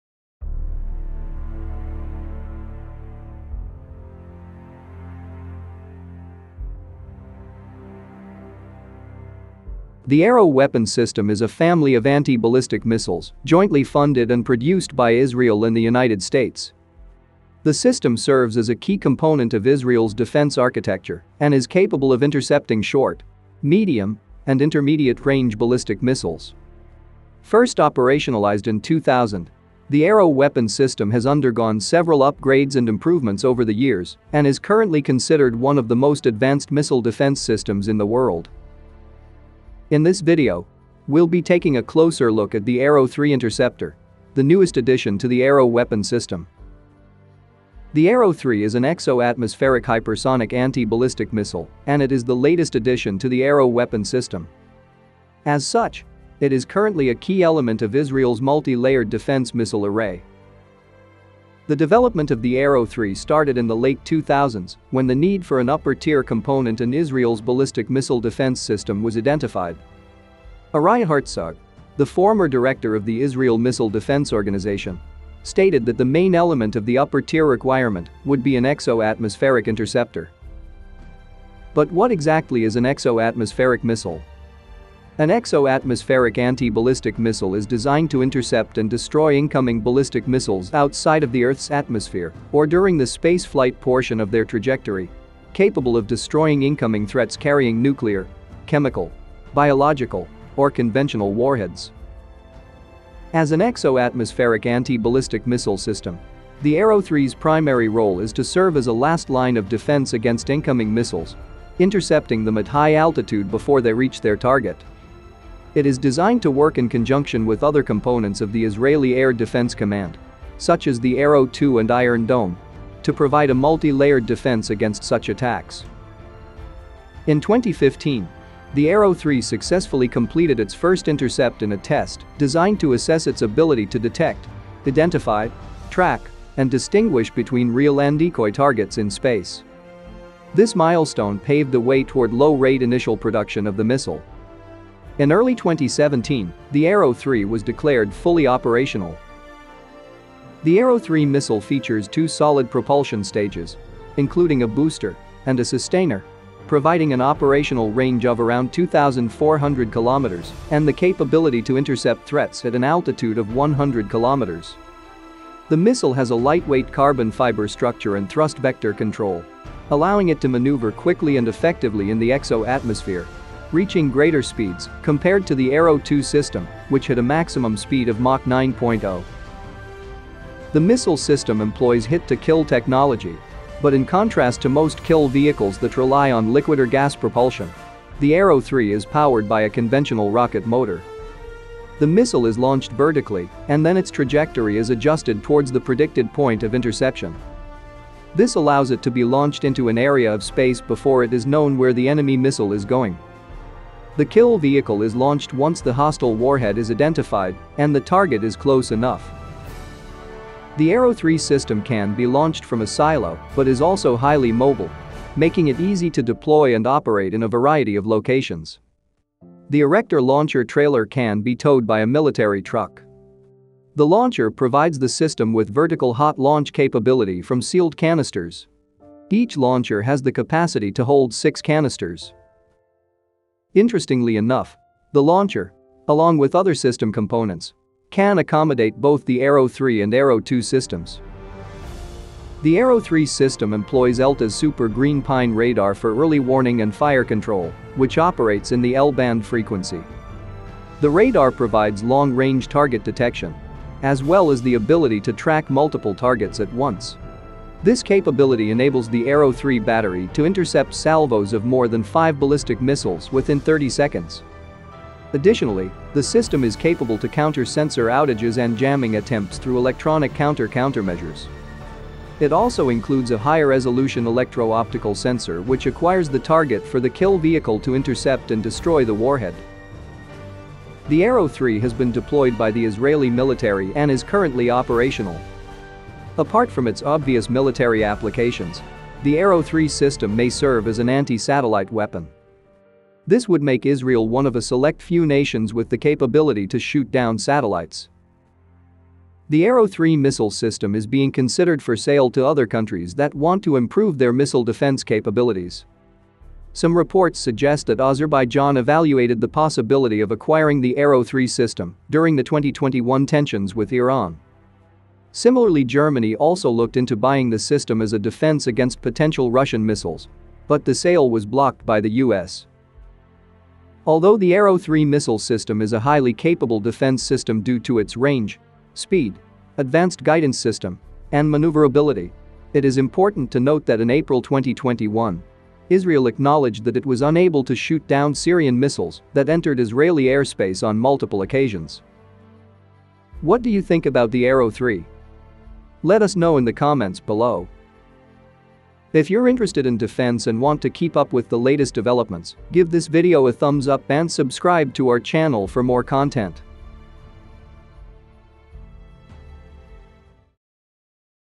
Arrow 3 Review: Israel's First Exoatmospheric Anti-Ballistic Missile
The Arrow Weapon System is a family of anti-ballistic missiles, jointly funded and produced by Israel and the United States. The system serves as a key component of Israel’s defense architecture and is capable of intercepting short, medium and intermediate-range ballistic missiles. It is currently considered one of the most advanced missile defense systems in the world.

In this video, we are taking a closer look at the Arrow 3 interceptor, the newest addition to the Arrow Weapon System.